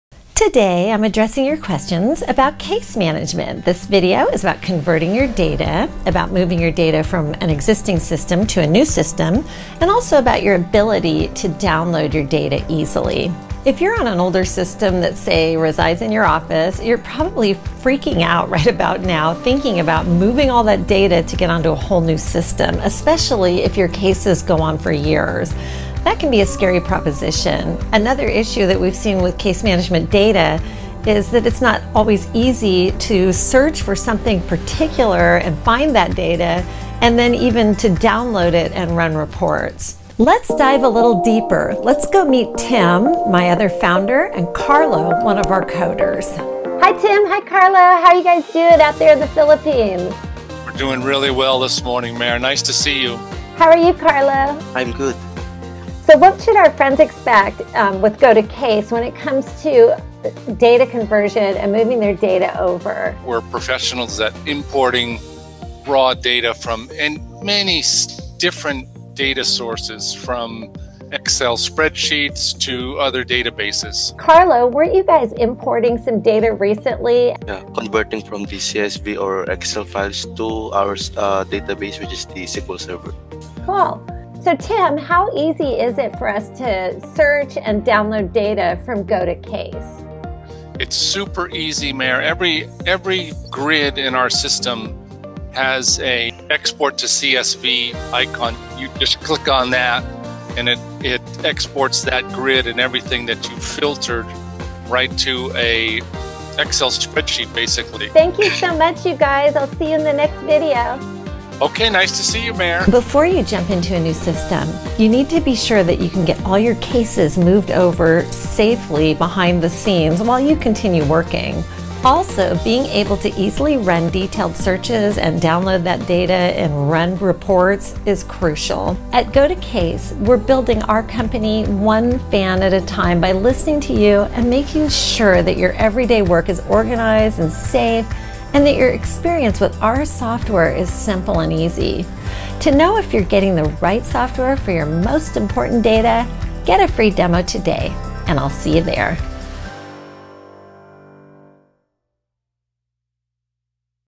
2. Moving Data to New Case Management
How can you move data to a new case management software, and also how easy is it to download our data at GoToCase.com?